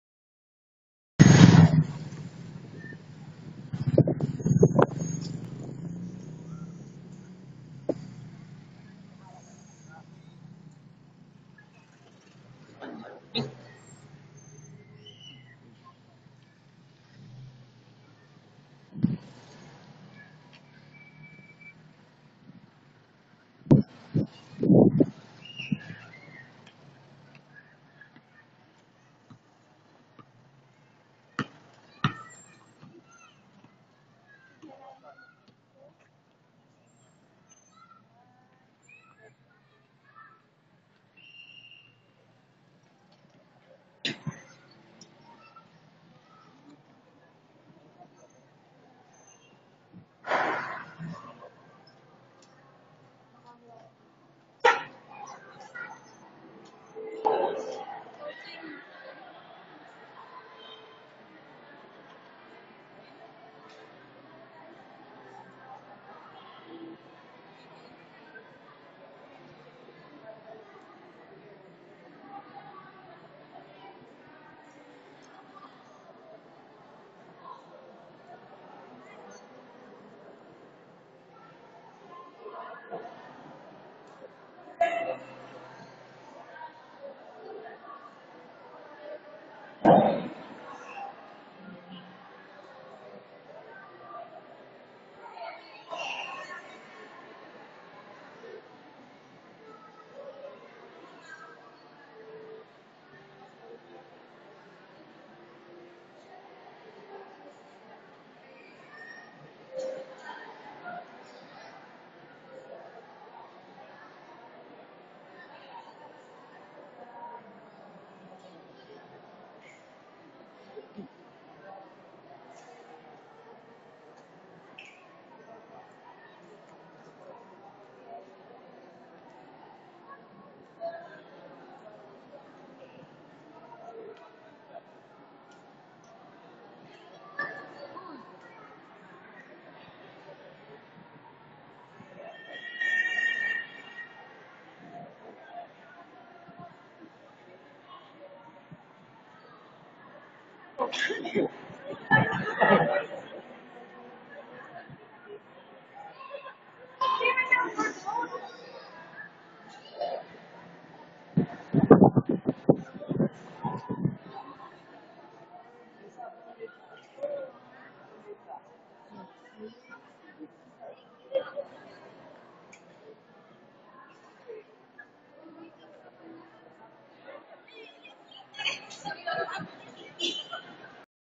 Mary Queen of Apostles Parish Tour
My first church tour of the year 2014. The Mary Queen of Apostles Parish located near Blessed Luisa School in Sucat, Parañaque City.
Video taken on January 12, 2014